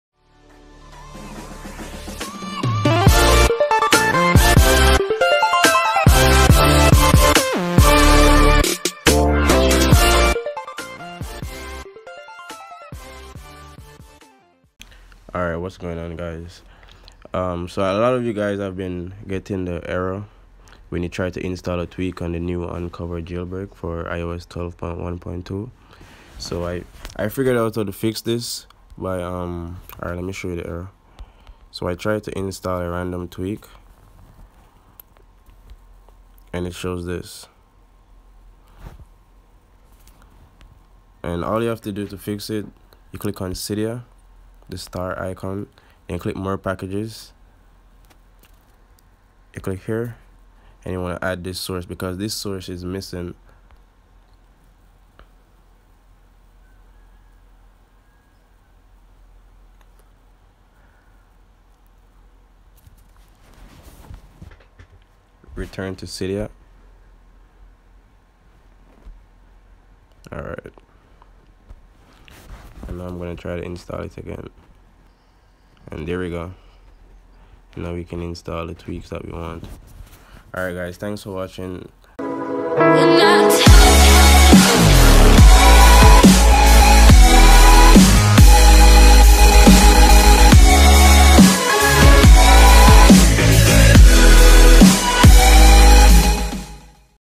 How To Fix: "The request modifications cannot be applied due to required dependencies" IOS 12
Tutorial on how to fix: "The request modifications cannot be applied due to required dependencies or conflicts that cannot be automatically found or fixed"

►Kik - QXUU
►Skype: live:Dev_HDD0

●▬▬▬▬▬▬ Songs▬▬▬▬▬▬●
●▬▬▬▬▬▬▬ Downloads ▬▬▬▬▬▬▬●
Link -
Link -